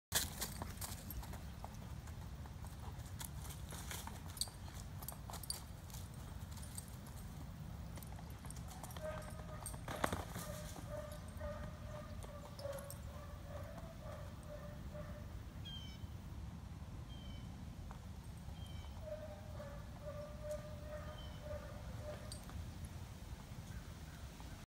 English Pointer with Lewis Rubber Dog Boots
First time to wear Lewis Rubber Dog Boots